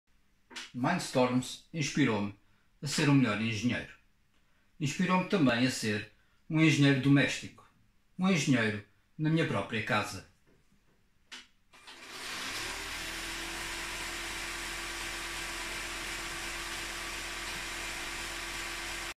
MINDSTORMS Inspired me
MINDSTORMS inspired me to be a better engineer.
It also inspired me to be a domestic engineer: an engineer at my own home.